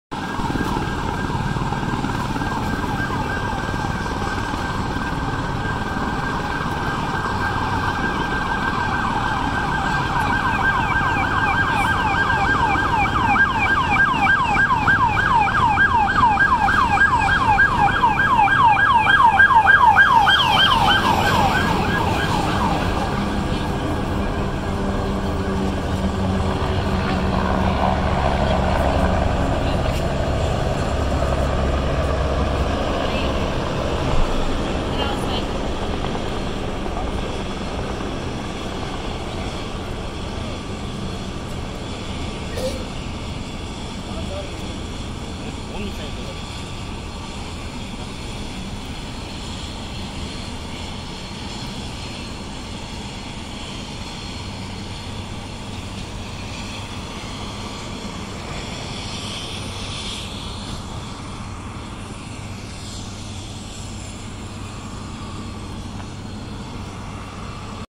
4th of July, busy Manhattan at Heliport, Pier 6
Pier 6, Heliport, New York City
at Holiday, Always we hear sirens. 🚨